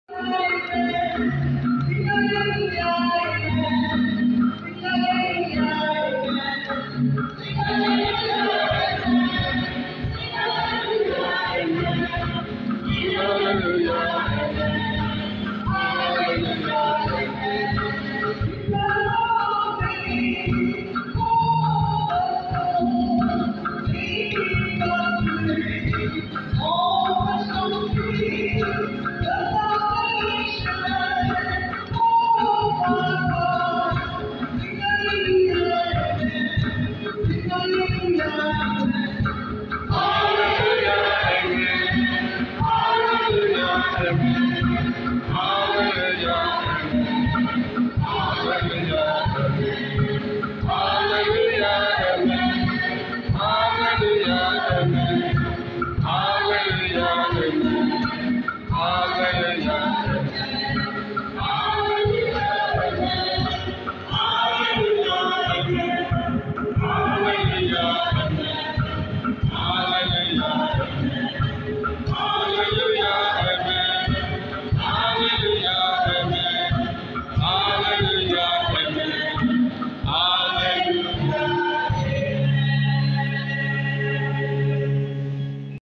Catholic Women Organization singing Gospel Acclamation during 2022 AGM Mass
When the Catholic Women Organization in Nigeria is in action, the Lord is praised!

Listen 👂 Enjoy Like 👏 and Share 💯 this video.